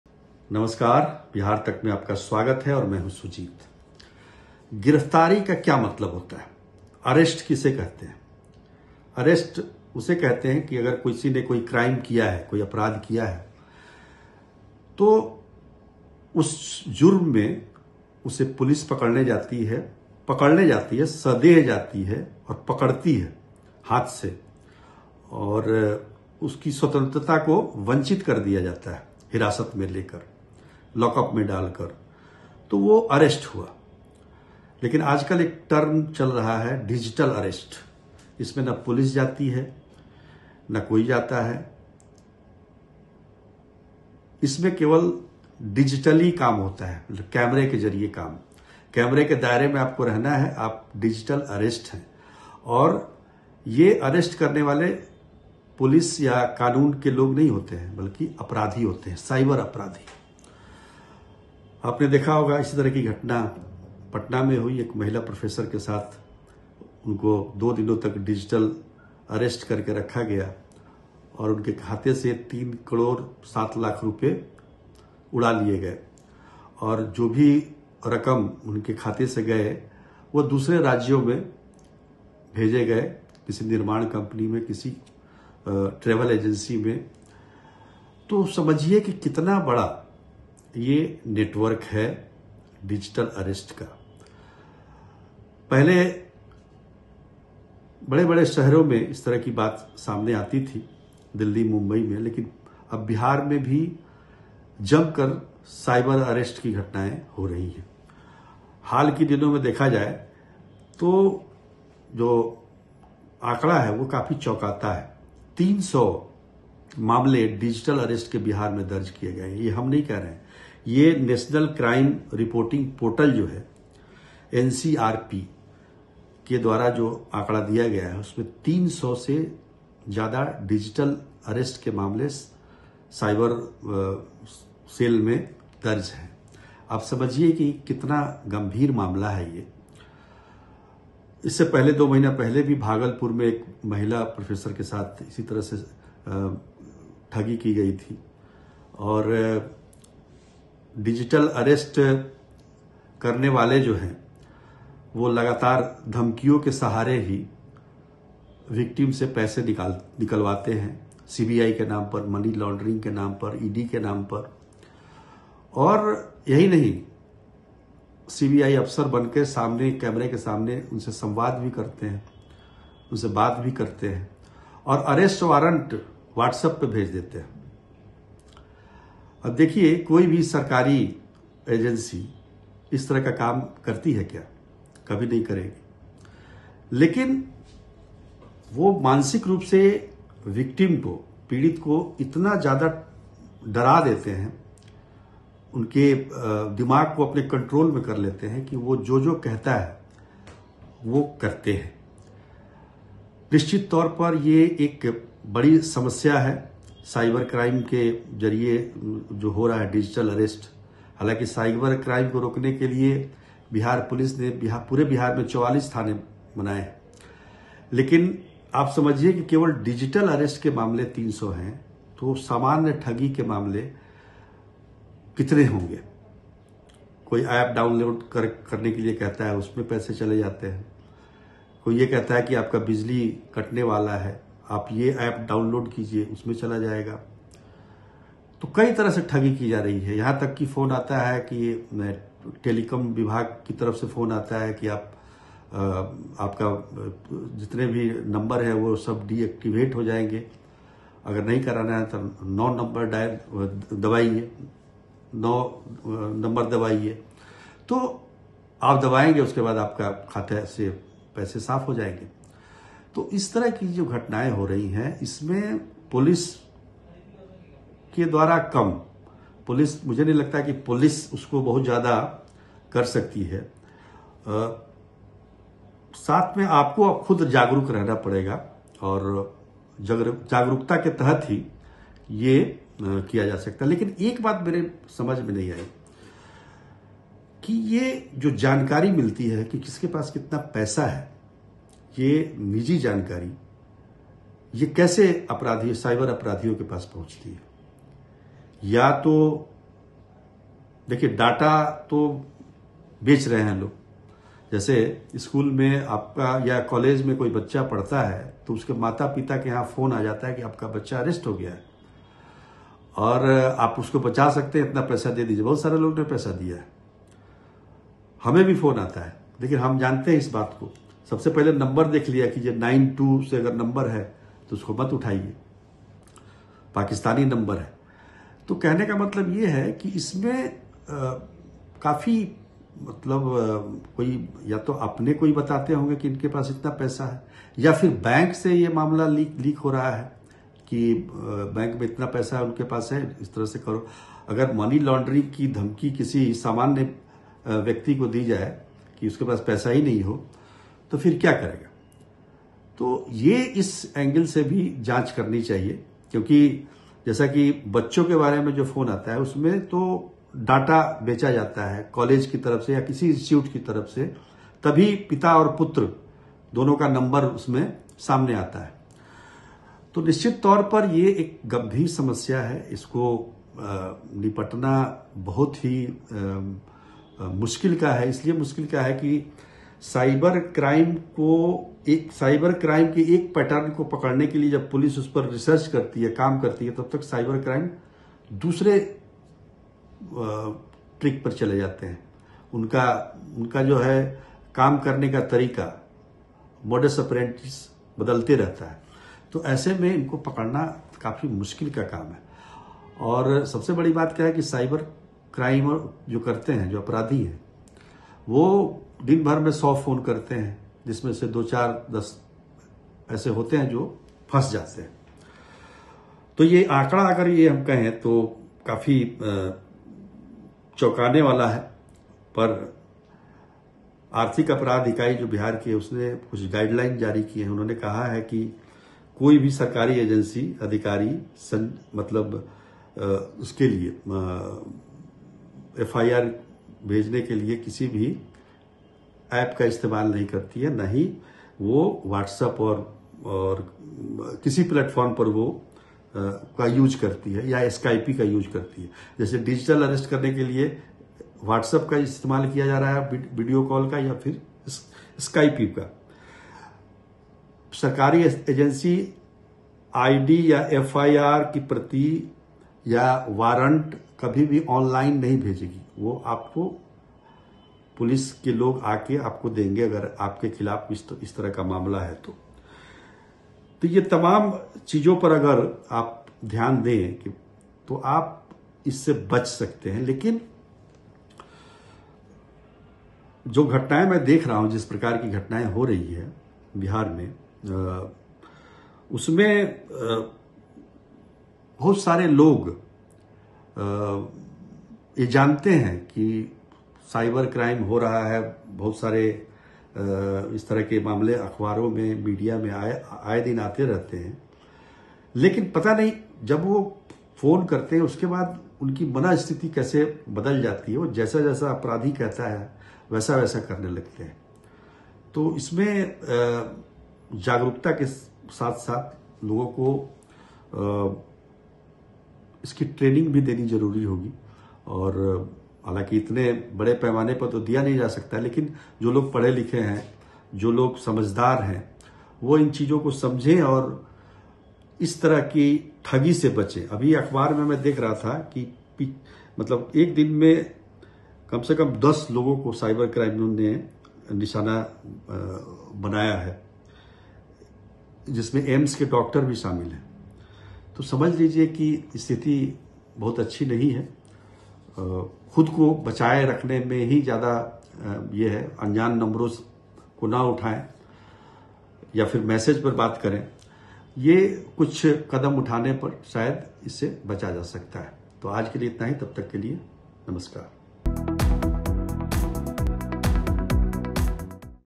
Explainer: Digital Arrest आप भी हो सकते हैं, बचने के लिए ये उपाय करें | Bihar Tak
बिहार में एक सेवानिवृत प्रोफेसर को 2 दिनों तक डिजिटल अरेस्ट करके रखा गया है... जानिए क्या होता है हाउस अरेस्ट और इससे बचने के लिए क्या उपया हैं ?